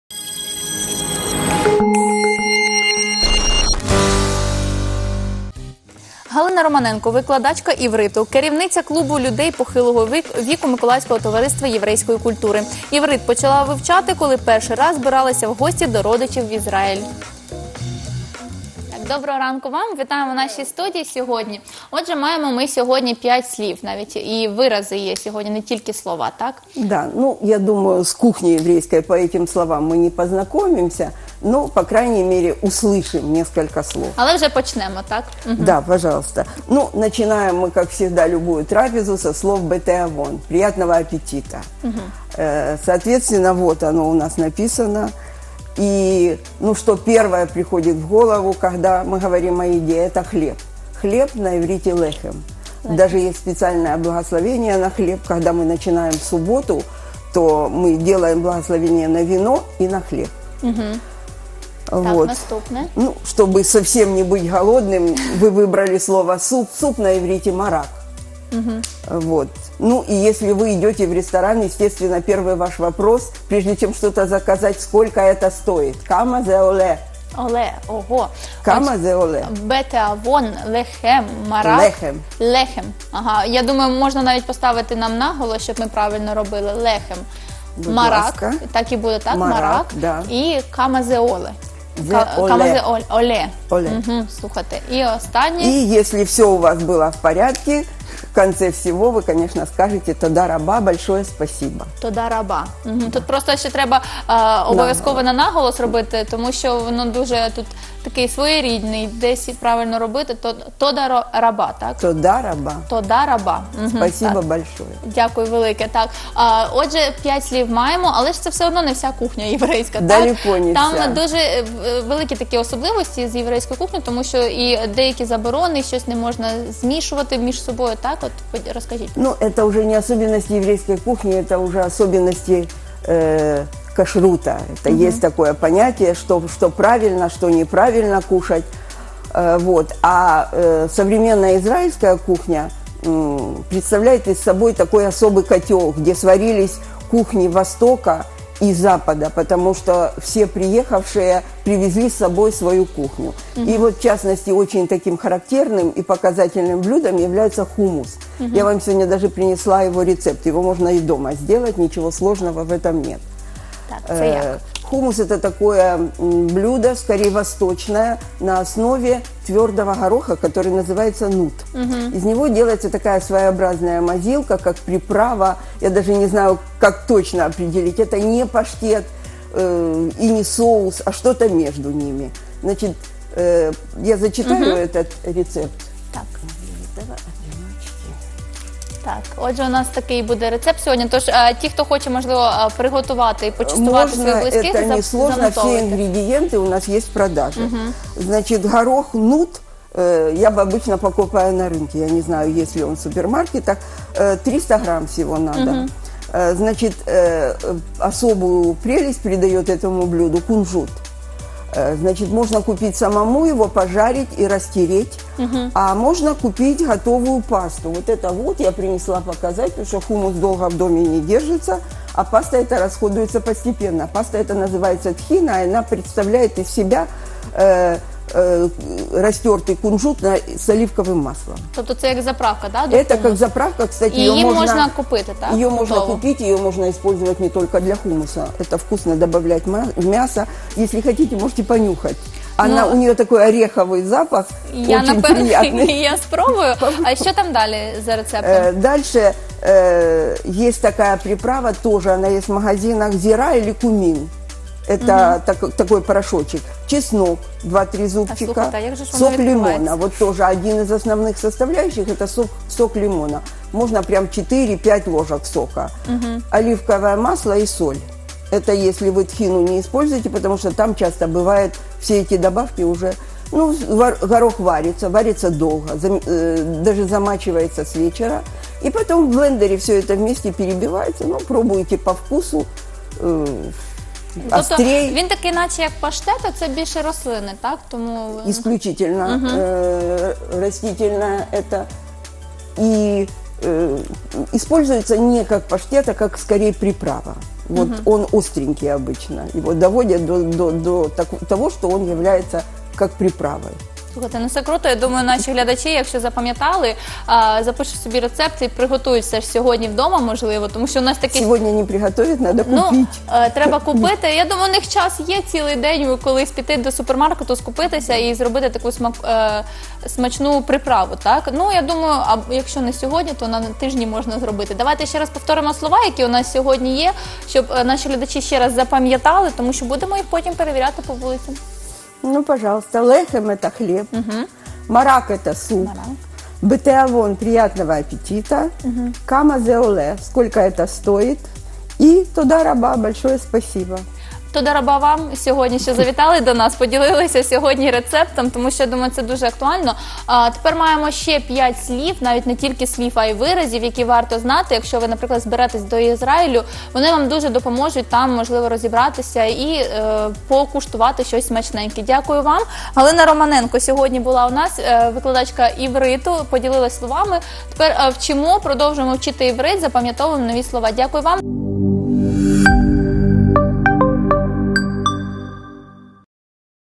19.11.2018. Новий день. Слово дня: іврит (їстівна тема)
У постійній рубриці «Слово дня» продовжуємо вчити іврит. Цього разу їстівна тема. Що означають לחם — ЛЕХЕМ, מרק — МАРАК, בתיאבון — БЕТЕАВОН , ?כמה זה עולה — КАМА ЗЕ ОЛЄ? , ארבה תודות — ТОДА РАБА?

Гостя: Галина Романенко, викладачка івриту.